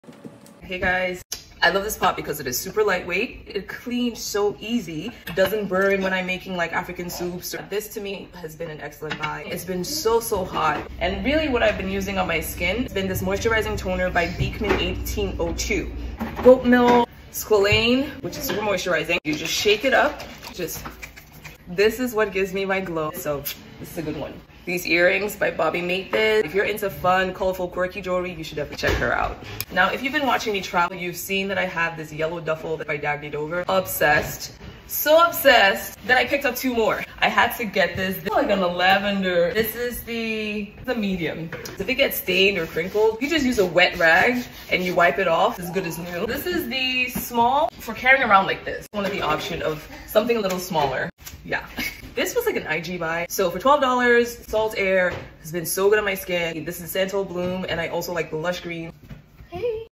RAPIDFIRE WEEKLY FAVES ROUNDUP
Every week, will share my best finds of the week. This week we have:

1. Caraway Dutch Oven: Lightweight, easy to clean, works amazing in high heat, doesn't burn, non-stick has lasted 2 years so far
2. Milk Shake Moisturizing Toner by Beekman 1802: leaves skin glowing and hydrated, lightweight and refreshing, perfect for these super hot months when we don't even want to wear makeup or heavy moisturizers
3. Earrings by Bobbi Made this: she's got a fun collection of colorful, one of a kind and fun jewelry
4. Dagne Dover duffels: come in amazing range of colors and sizes, easy to clean, great for travel and more
5. Saltair Body washes: spotted on IG and happy I did, for $12 a bottle, you get sophisticated botanicals, with skin moisturizing ingredients. Love the look of the aluminum bottle too